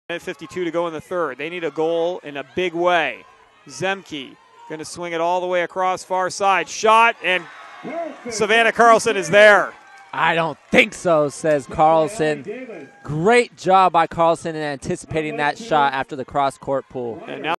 Savannah Carlson Save
Savannah Carlson saves a shot as Valhalla attempts to comeback as they trail 7-2.